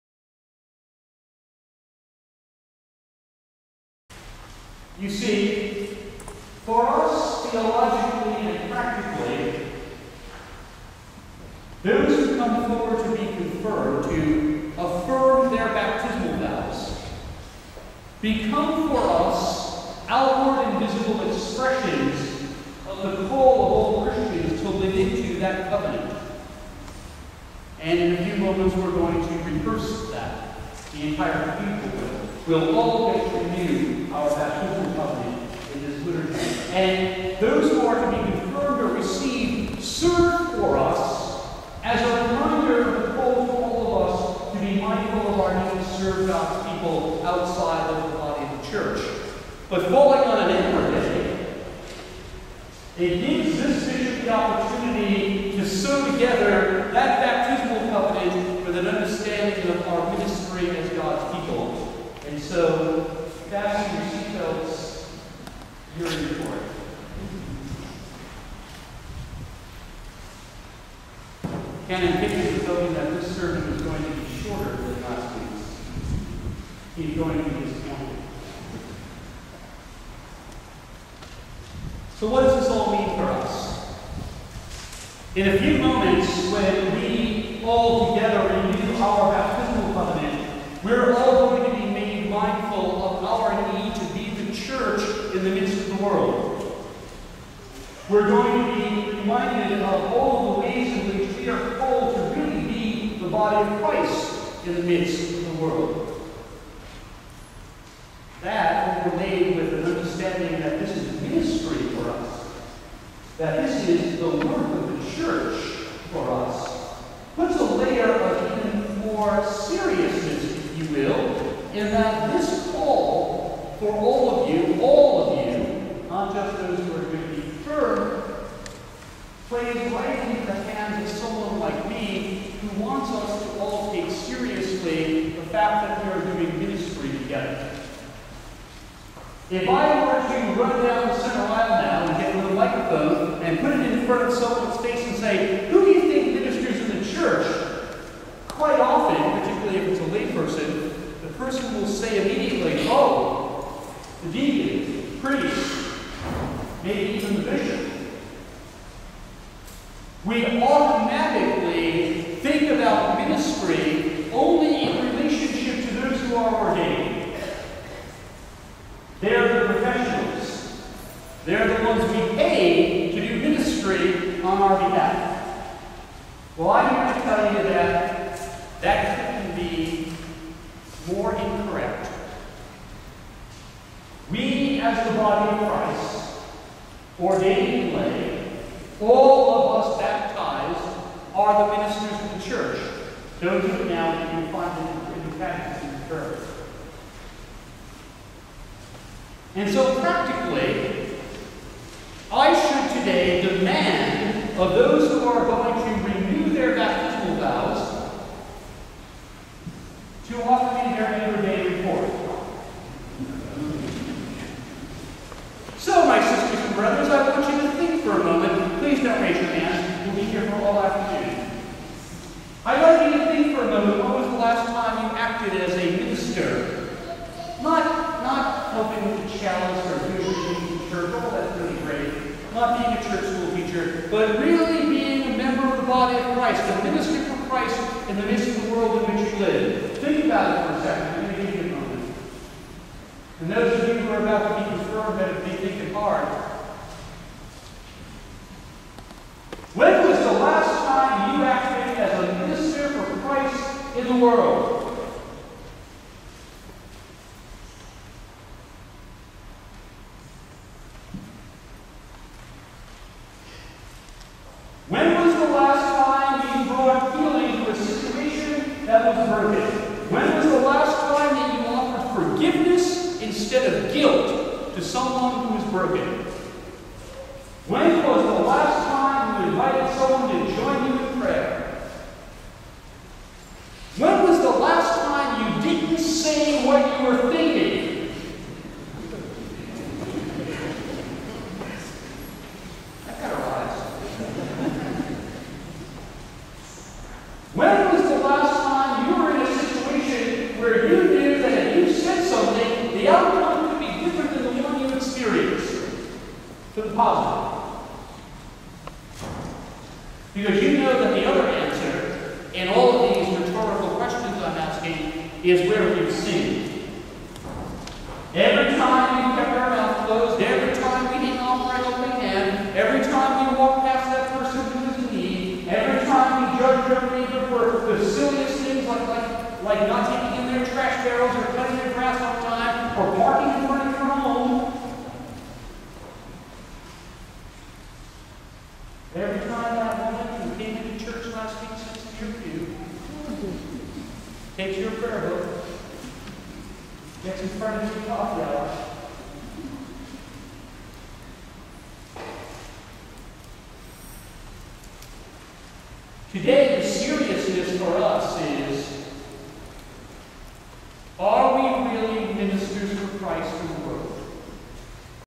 Confirmation at the Cathedral of the Incarnation- June 18,2011
Part of Bishop Provenzano's sermon preached at the Cathedral on Saturday, June 18, 2011. Some of the Confirmands were from St Joseph's Episcopal Church in Queens Village.